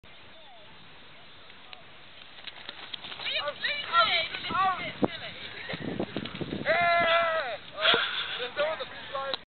40m hurdles final
liam frazer + some guy run the 40m hurdles at greenbelt 2008 olympics